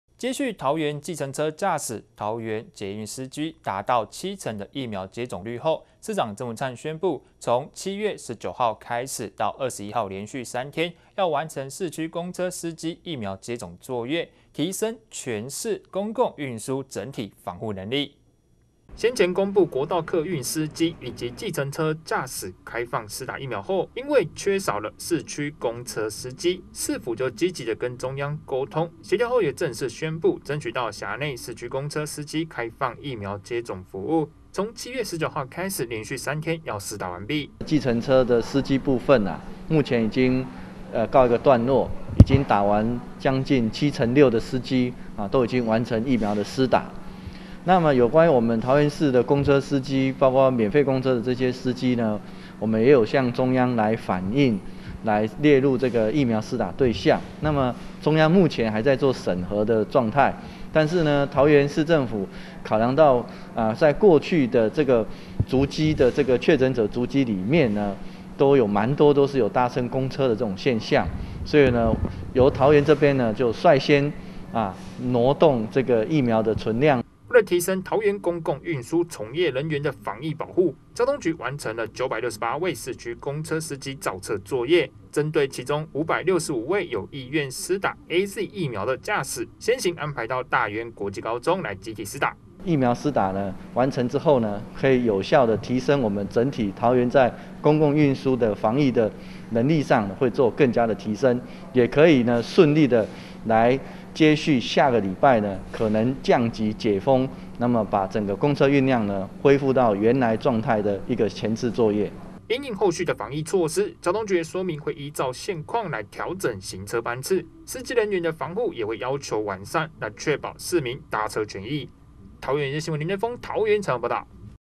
桃園有線新聞20210721-市區公車司機開打疫苗 提升公共運輸防護力
接續桃園計程車駕駛、桃園捷運司機達到七成的疫苗接種率後，市長鄭文燦宣布，自7月19號開始到21號連續3天，要完成市區公車司機疫苗接種作業，提升全市公共運輸整體防護能力。